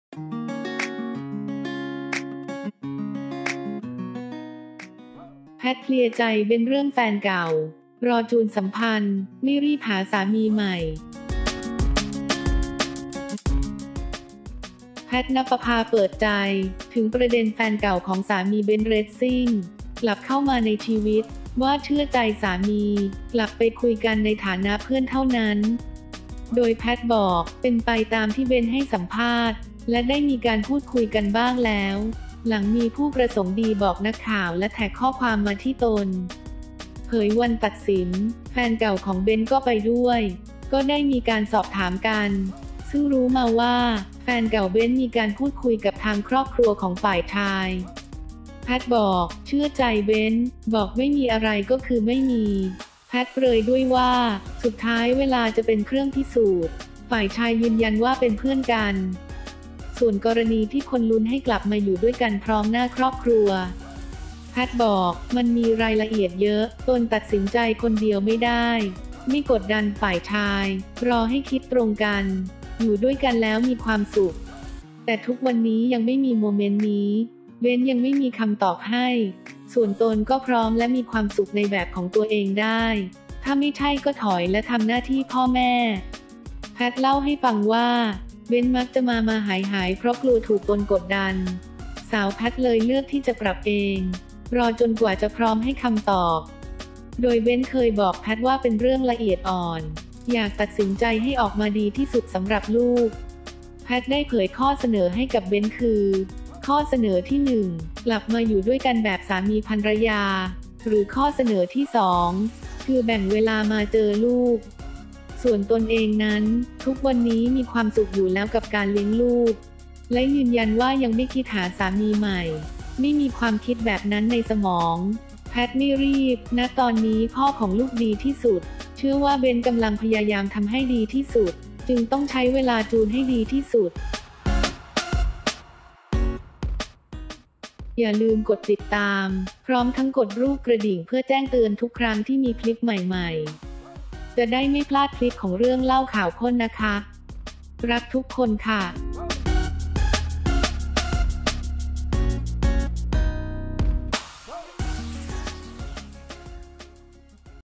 แพท ยื่นข้อเสนอ 2 ข้อ! เบนซ์ จะเอายังไง เผย รอจูนสัมพันธ์ ไม่รีบหาผัวใหม่
แพท ณปภา เปิดใจถึงประเด็นแฟนเก่าของสามี เบนซ์ เรซซิ่ง กลับเข้ามาในชีวิต ว่าเชื่อใจสามีกลับไปคุยกันในฐานะเพื่อนเท่านั้น โดยแพทบอกเป็นไปตามที่เบนซ์ให้สัมภาษณ์ได้มีการพูดคุยกันบ้างแล้ว หลังมีผู้ประสงค์ดีบอกนักข่าว และแท็กข้อความมาที่ตน เผยวันตัดสินแฟนเก่าของเบนซ์ก็ไปด้วย ก็ได้มีการสอบถามกัน ซึ่งรู้มาว่าแฟนเก่าเบนซ์มีการพูดคุยกับทางครอบครัวของฝ่ายชาย เชื่อใจเบนซ์บอกไม่มีอะไรก็คือไม่มี เปรยสุดท้ายเวลาจะเป็นเครื่องพิสูจน์ ฝ่ายชายยืนยันว่าเป็นเพื่อนกัน